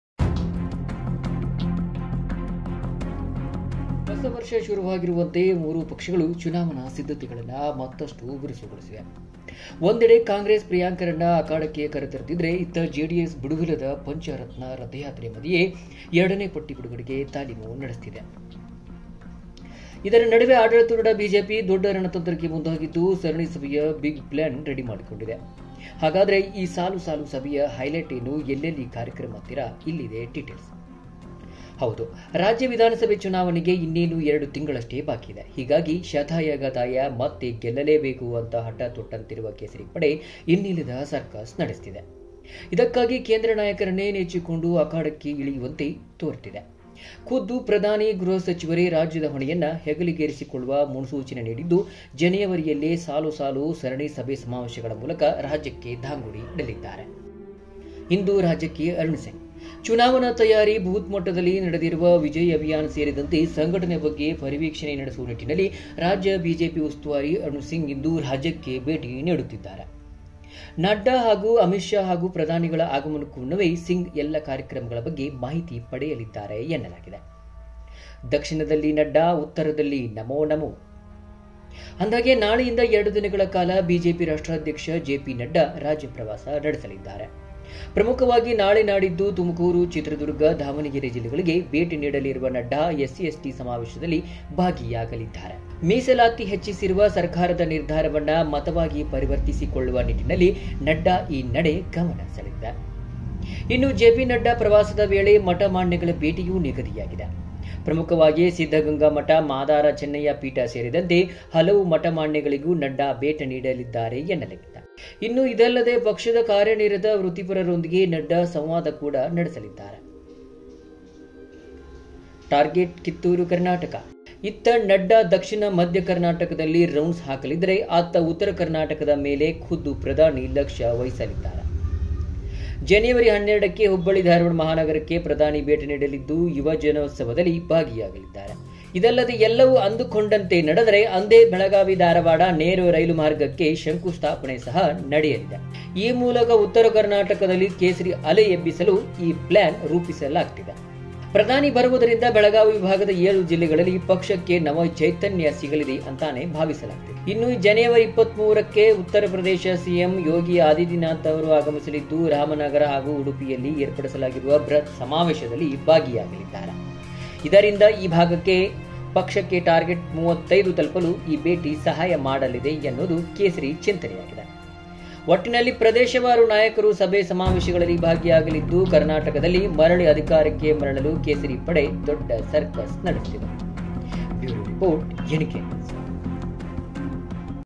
Karnataka Assembly Election 2023 - ಮೂವರು ನಾಯಕರು..ನೂರು ಲೆಕ್ಕಾಚಾರ.! ಏನಿದು ಸರಣಿ ಸಭೆ Strategy.?
ಹೊಸ ವರ್ಷ ಶುರುವಾಗಿರುವಂತೆ ಮೂರು ಪಕ್ಷಗಳು ಚುನಾವಣಾ ಸಿದ್ಧತೆಗಳನ್ನು ಮತ್ತಷ್ಟು ಬಿರುಸುಗೊಳಿಸಿವೆ. ಒಂದೆಡೆ ಕಾಂಗ್ರೆಸ್ ಪ್ರಿಯಾಂಕಾರನ್ನು ಅಖಾಡಕ್ಕೆ ಕರೆ ತರ್ತಿದ್ರೆ ಇತ್ತ ಜೆಡಿಎಸ್ ಬಿಡುವಿಲ್ಲದ ಪಂಚ ರತ್ನರಥಯಾತ್ರೆ ಮಧ್ಯೆಯೇ 2ನೇ ಪಟ್ಟಿ ಬಿಡುಗಡೆಗೆ ತಾಲೀಮು ನಡೆಸ್ತಿದೆ. ಇದರ ನಡುವೆ ಆಡಳಿತಾರೂಢ ಬಿಜೆಪಿ ದೊಡ್ಡ ರಣತಂತ್ರಕ್ಕೆ ಮುಂದಾಗಿದ್ದು ಸರಣಿ ಸಭೆಯ ಬಿಗ್ ಪ್ಲ್ಯಾನ್ ರೆಡಿ ಮಾಡಿಕೊಂಡಿದೆ.